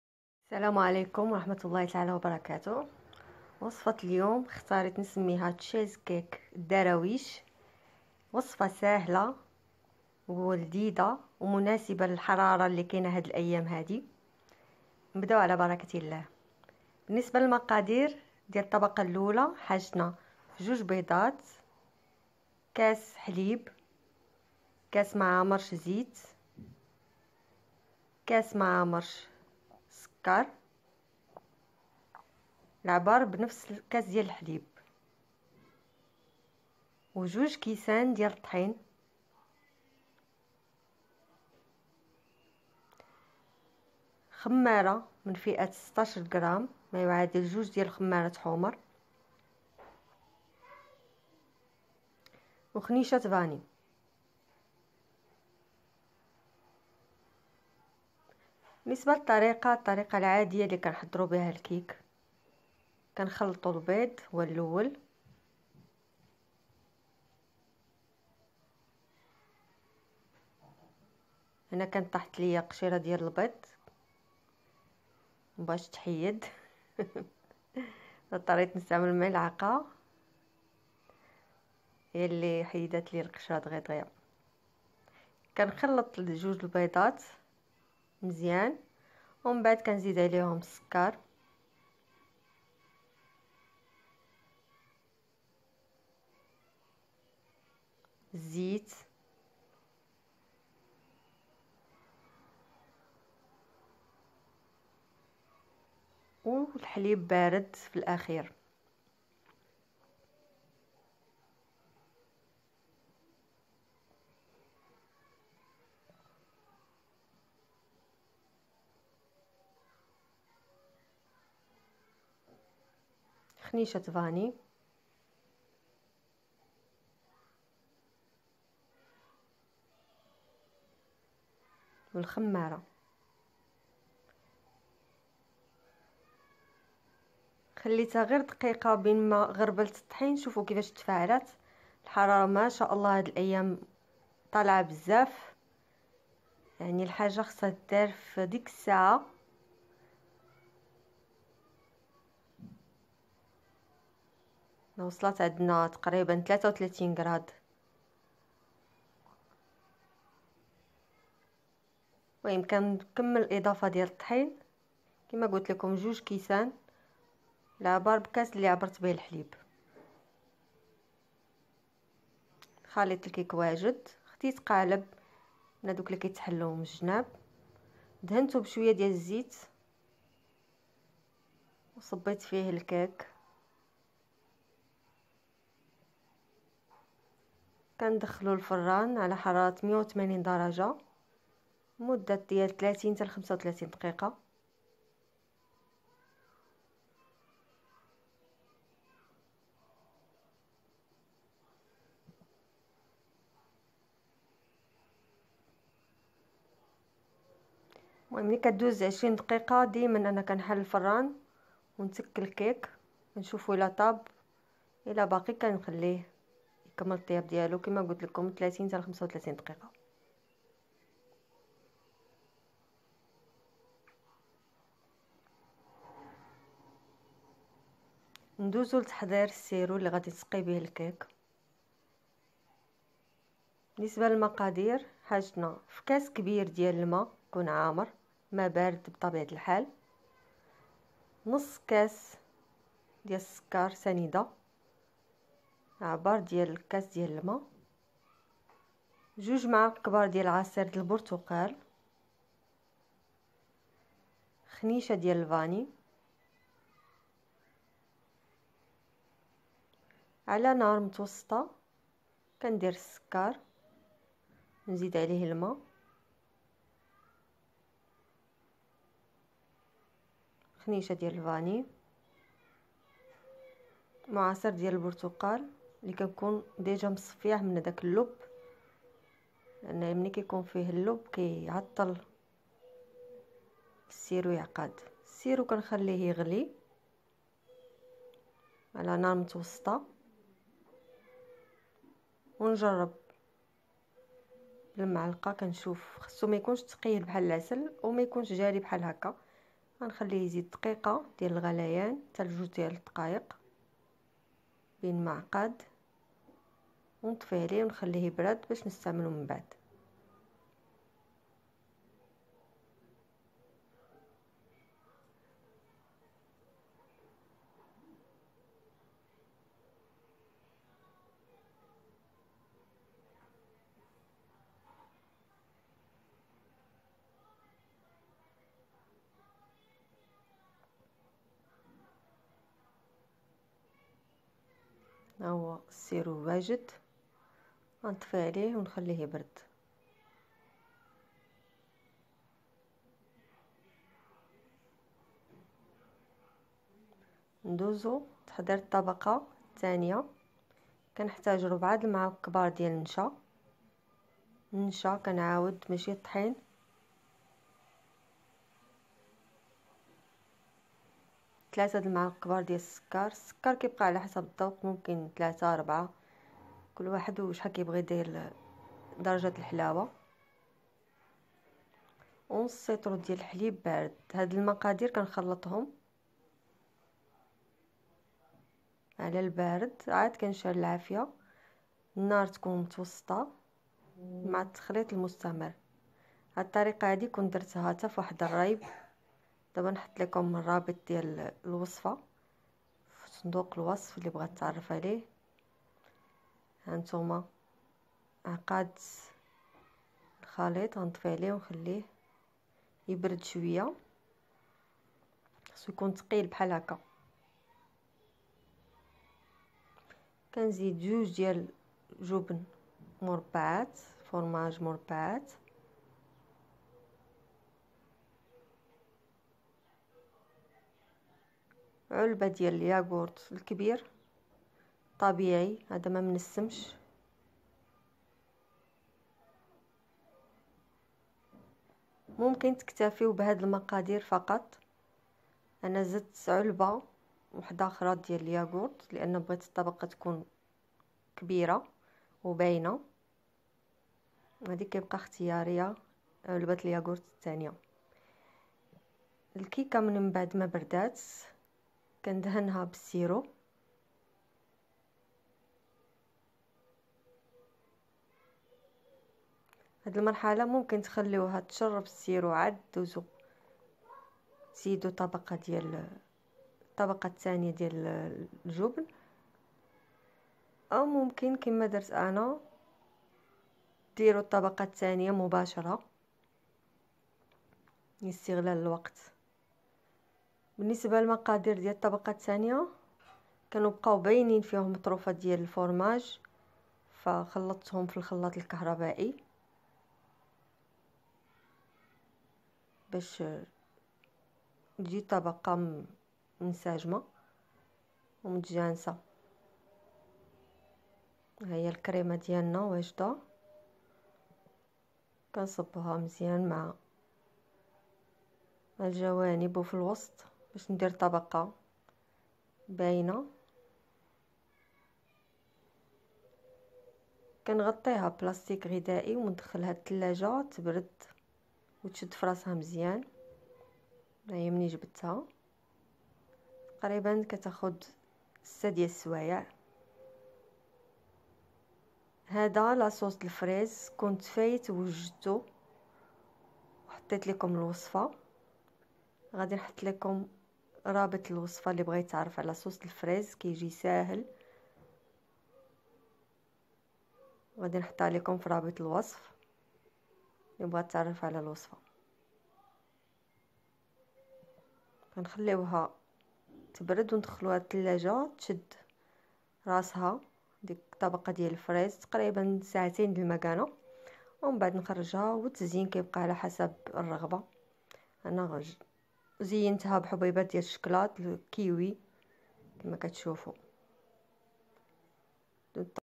كيكة الدراويش الباردة تشيزكيك الدراويش بدون فيلاديلفيا ولا جيلاتين
كيكة الدراويش الباردة في متناول الجميع للتعرف على المقادير والطريقة المرجو مشاهدة كاملة للفيديو